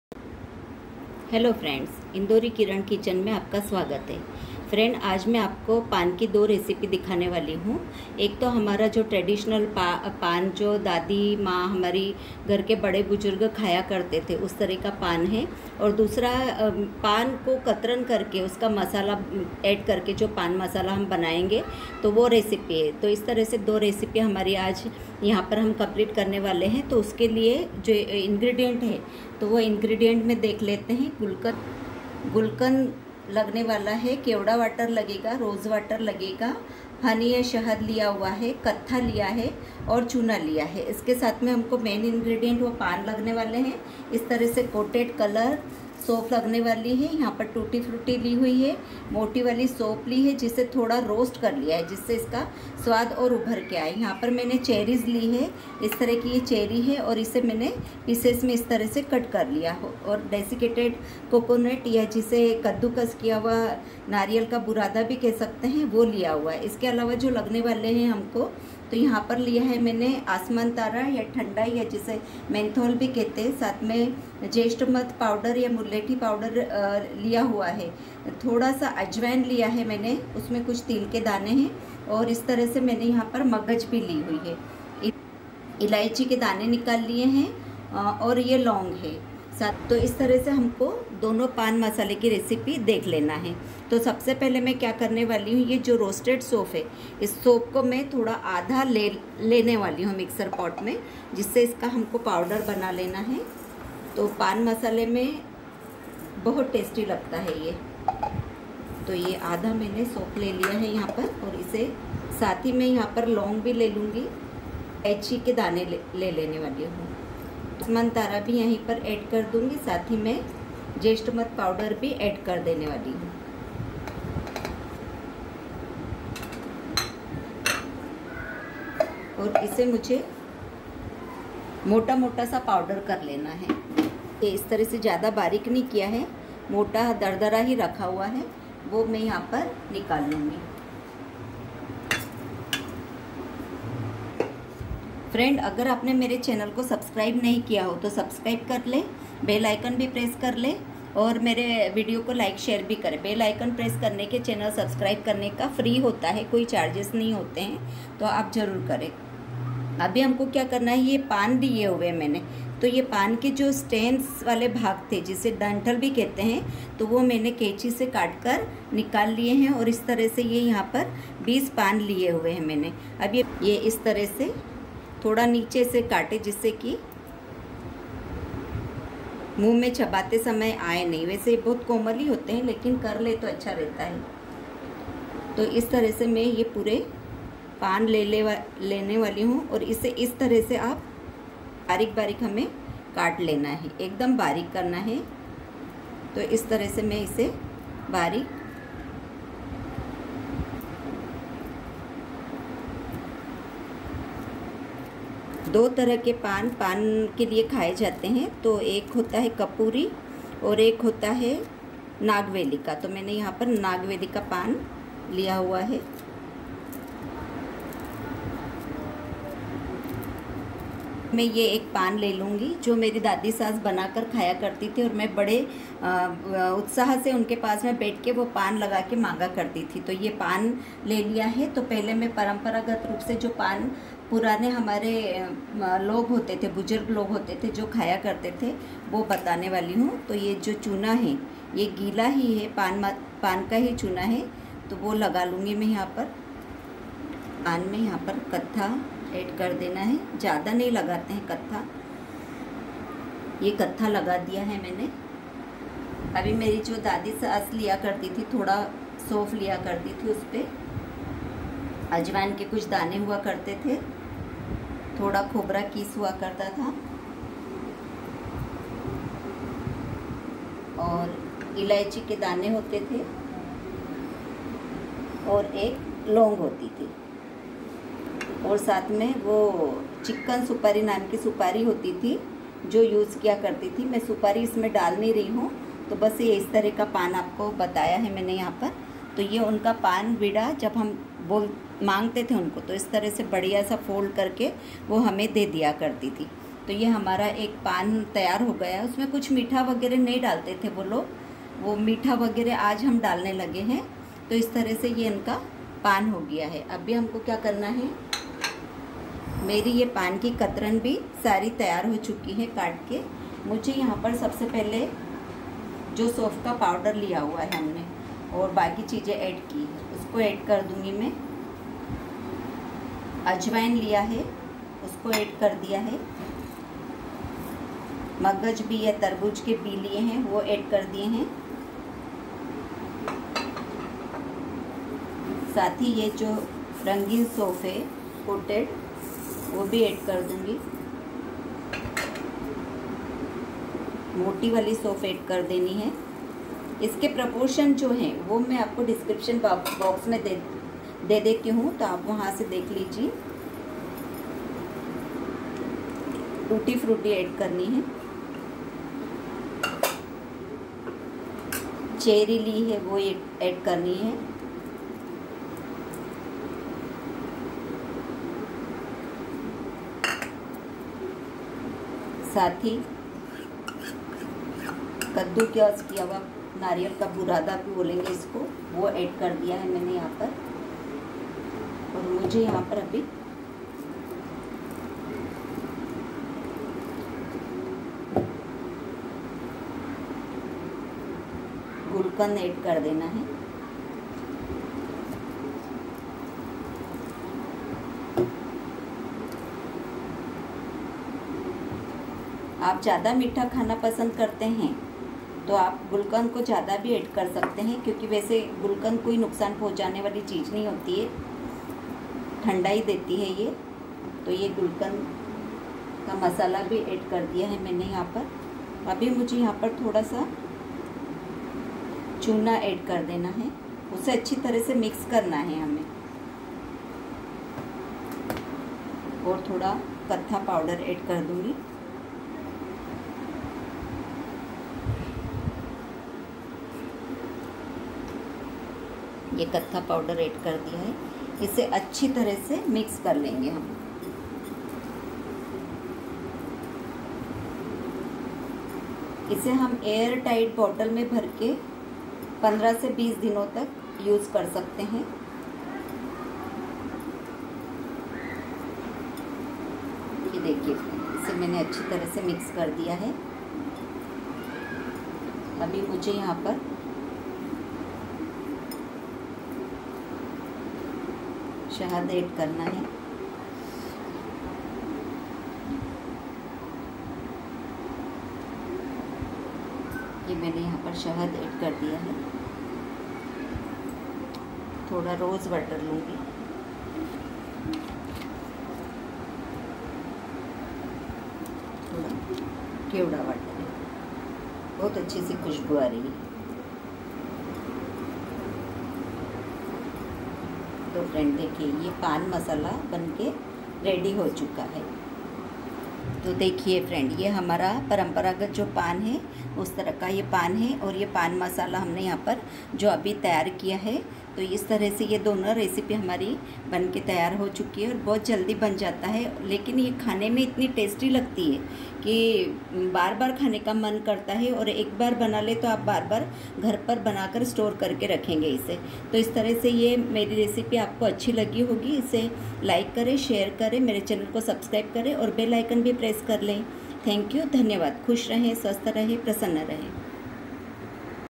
pan - masala | पान - पान मसला |बनाये अब घर पर स्वाद ले बाजार का
pan- pan masala | पान - पान मसाला | बनाये अब घर पर स्वाद ले बाज़ार का | 🤤🤤🤤🤤🤤😛😛😛💚💚💚💚🌱🌱🌱🌱🌱🌱🌱🌱🌱🌱🌱🌱
betel leaf (pan) is favourite in indian subcontinent
ingredients :- betel leaves -20  -pan patte
roasted fennel seeds - 40 gm
tutti-  fruiti - 25 gm
Cherry - 20 - 25
ketehu - pow  - 4 - 5 gm- kathha
lime  wet - 4-5 gm - gila chuna
sweet rose petals - 3 tbs - chillan
ros water - 1 tsp - gulab jal
mewada water - 1tsp
menthol - 2 thread - thandai
green cardamom -6-7 - hari elaichi
cloves - 6-7 - lavang
honey - 3 the
link of my other masala video
masala kaju
my all recipes available on YouTube chennal